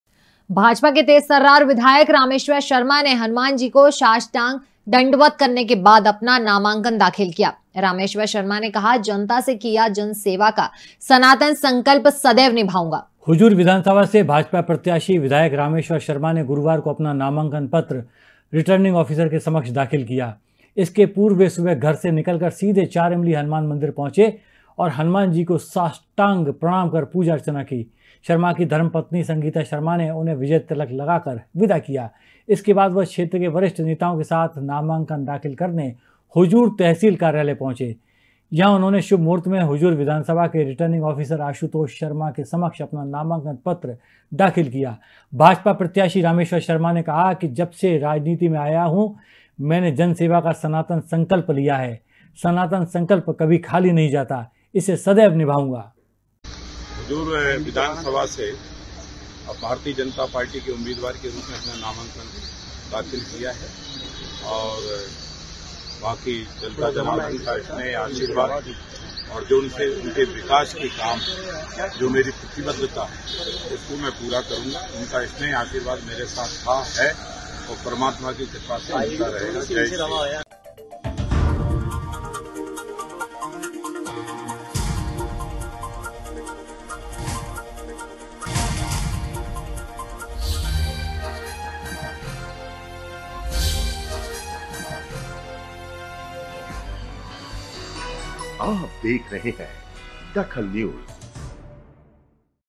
DAKHAL NEWS विधायक रामेशवर शर्मा ने हनुमानजी को साष्टांग दंडवत करने के बाद किया नामांकन दाखिल
विधायक रामेशवर शर्मा ने हनुमानजी को साष्टांग दंडवत करने के बाद किया नामांकन दाखिल